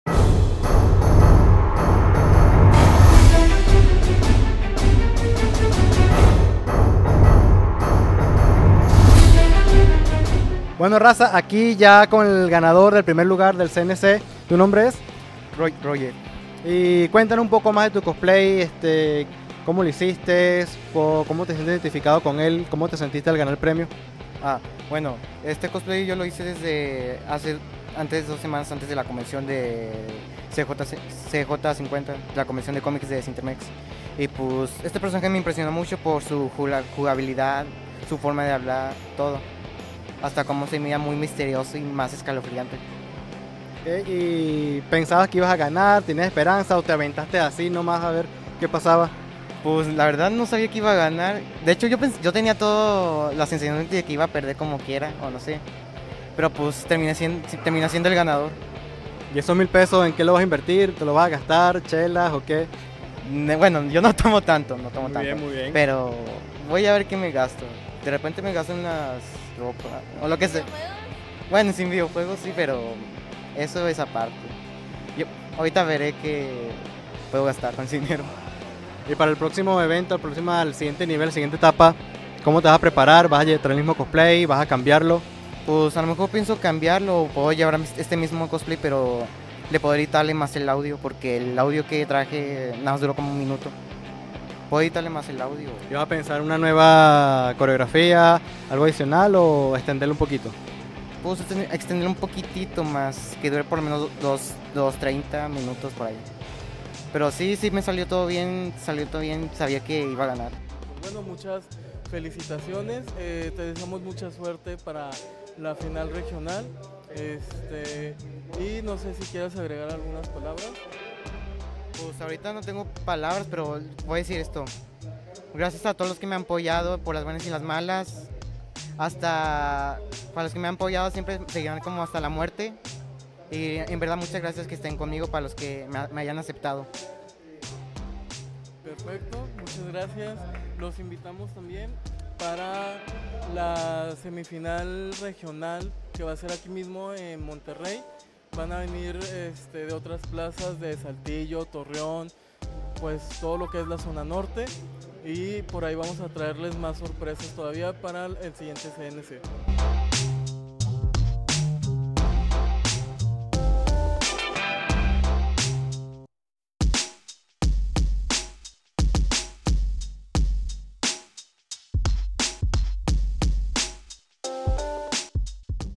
Roger Zapata Ganador del Concurso de Nacional de Cosplay ( Nuevo León )
Aquí el Ganador Concurso de Nacional de Cosplay 13 de mayo del 2017 organizado por la FrikiPlaza en conjunto con la Expo Toy Machine Monterrey